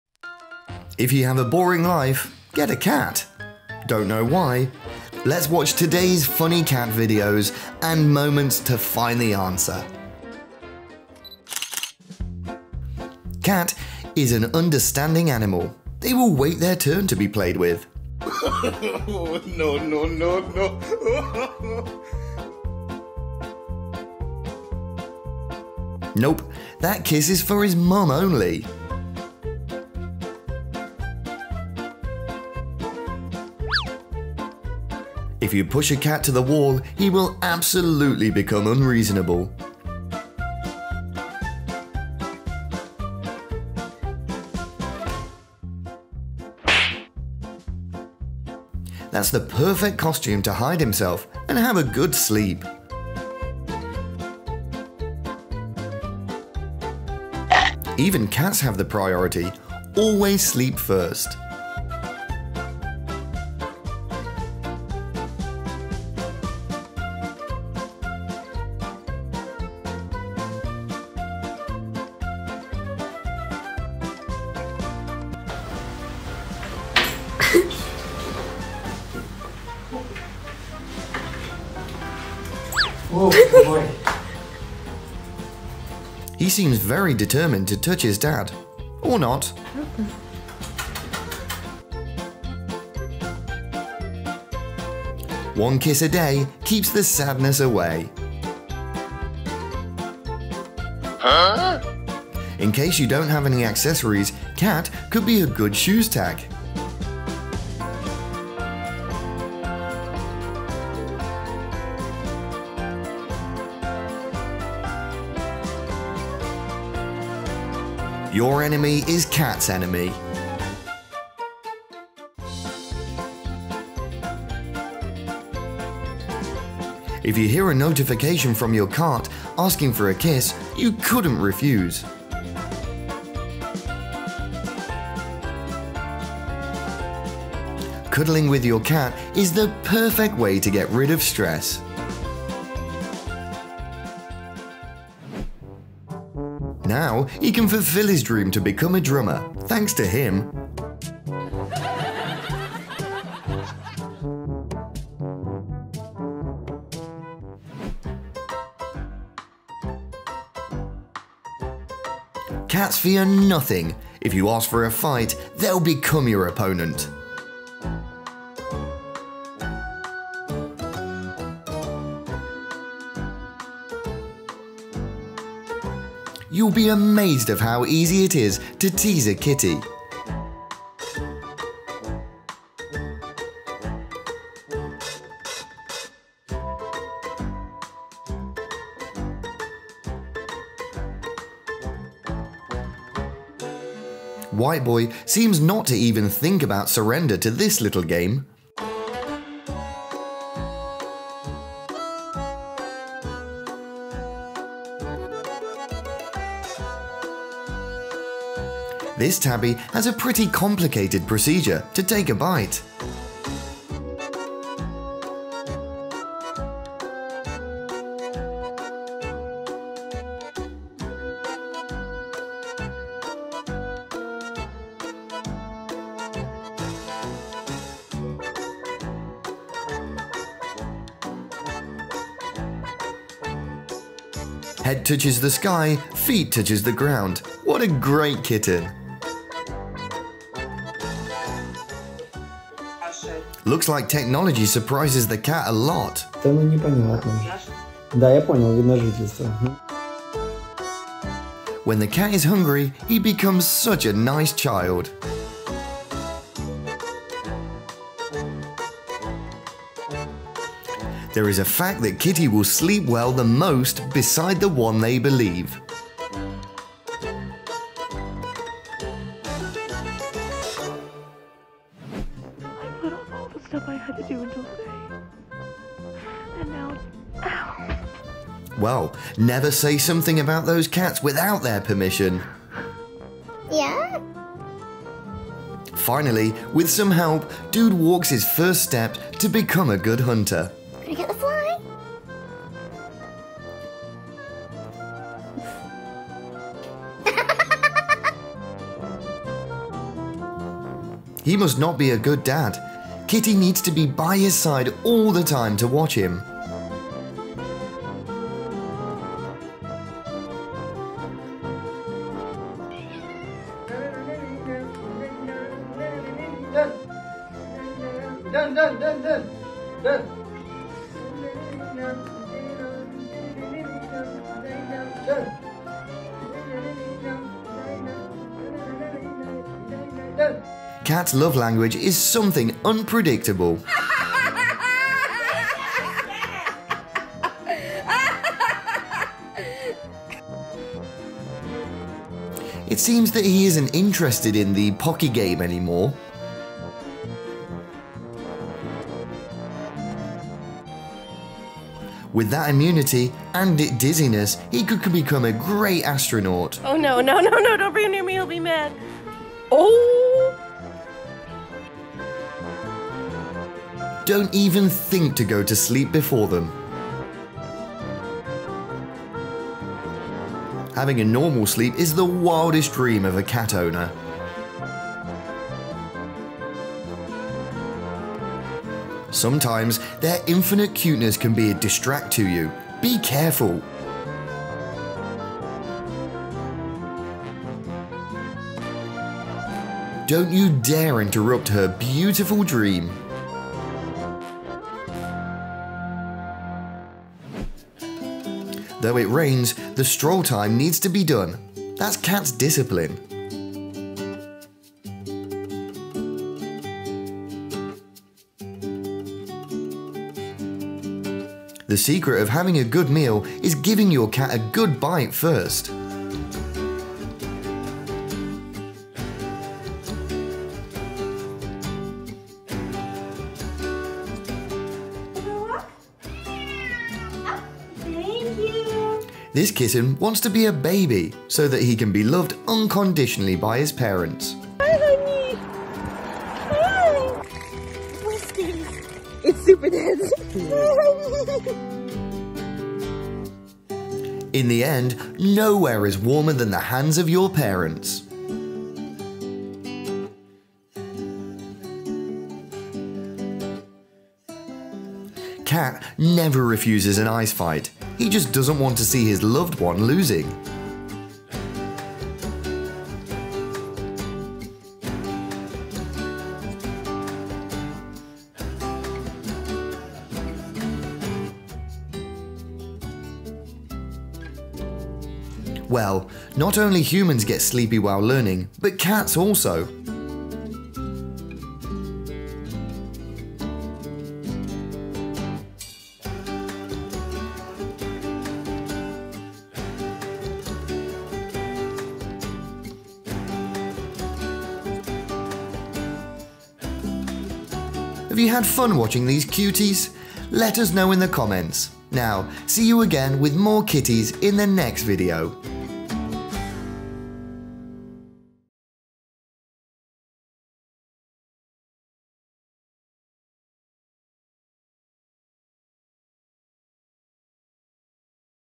Moment When You Realizes Your Cat Really Does Love You - Cute Animal Show Love
If you have a boring life, get a cat. Don't know why? Let with us watch these funny cat moments and find the answer

Most Emotional Dogs Reunions with Their Owners That Will Melt Your Heart ❤️Best of video 2022